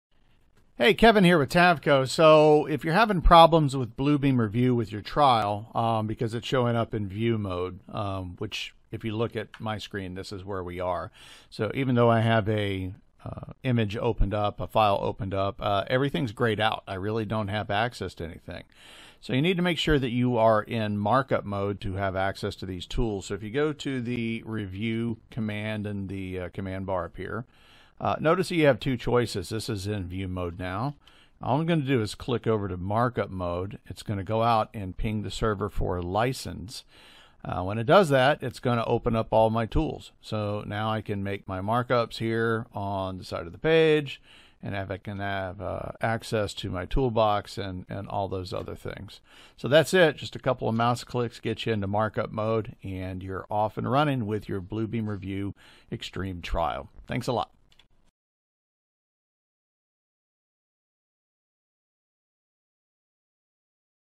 Bluebeam - Switching Between View and Markups Modes - TAVCO
Sometimes new Bluebeam Revu users are frustrated because they are in "View Mode" and don't have access to any tools. No worries. There is a quick solution to fix this. In this video learn how to quickly switch between the modes.